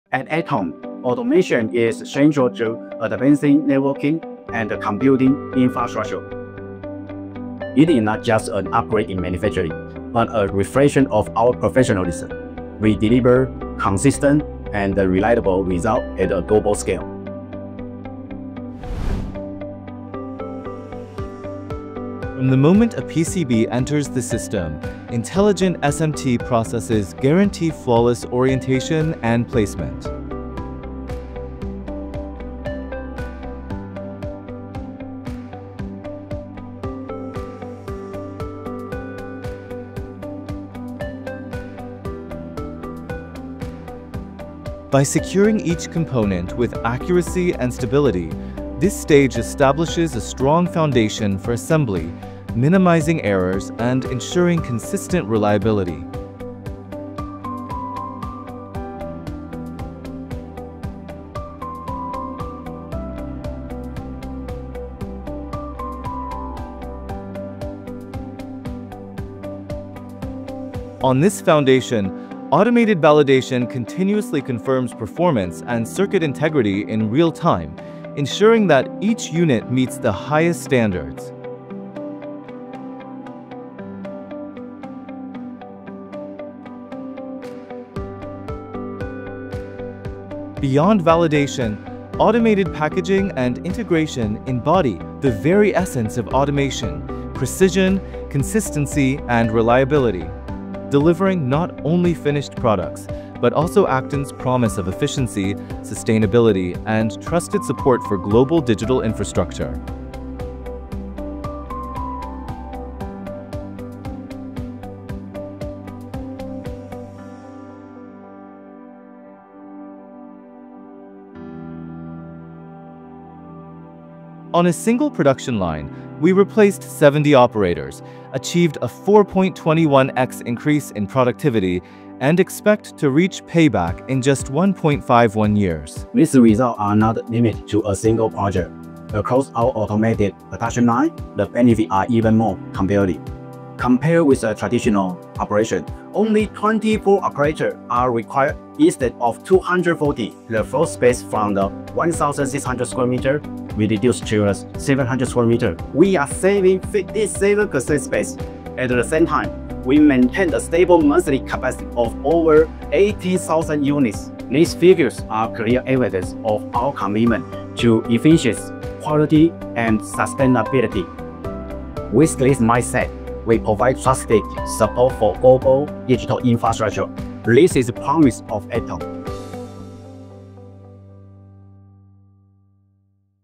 The Manufacturing Line That Never Sleeps
Step inside Accton’s state-of-the-art manufacturing line, where innovation never rests, precision drives every process, and quality is built into every detail. From design to production, we ensure every solution meets the world’s growing demand for high-performance, energy-efficient network infrastructure.

At Accton, we’re powering the future of connectivity with technology that never sleeps.